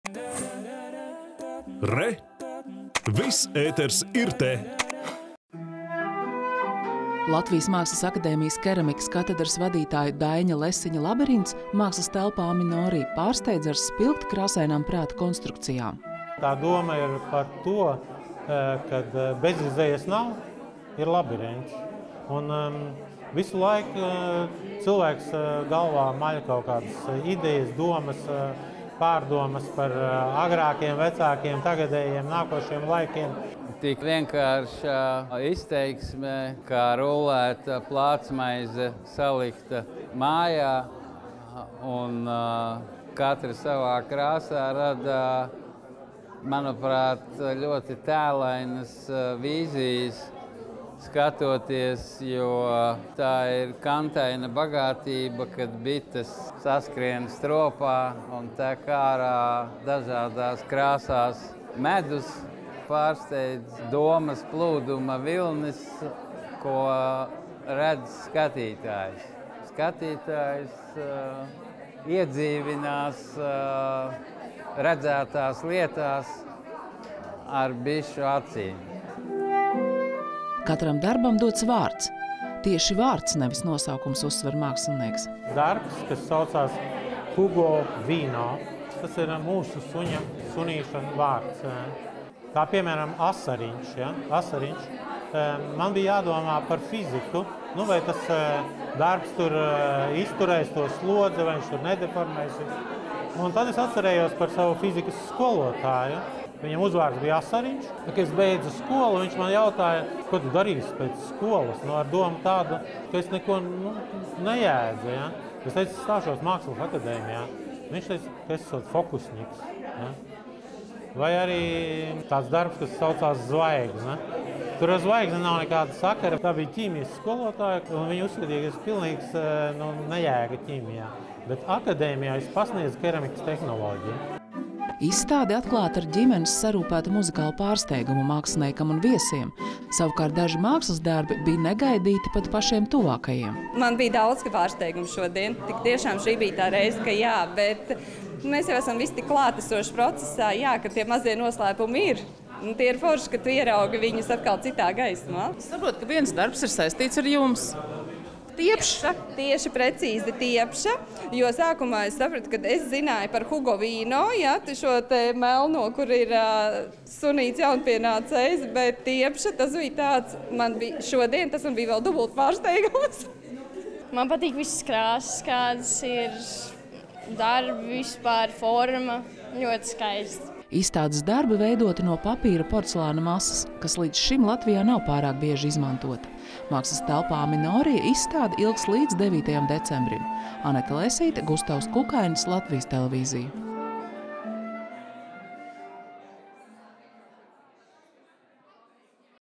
Keramiķa Daiņa Lesiņa trauslais konstrukciju “Labirints”
Mākslas telpa un grāmatnīca “Aminori” regulāri rīko izstādes, skatītāju prātos atklājot jaunu telpu domas lidojumam. Mūsu filmēšanas grupa vakar vakarā bija klāt brīdī, kad tika atvērta jauna izstāde – keramiķa un Latvijas Mākslas akadēmijas Keramikas katedras vadītāja Daiņa Lesiņa personālizstāde “Labirints”. Izstādes atklāšanas iespaidi -  Anetes Lesītes sižetā.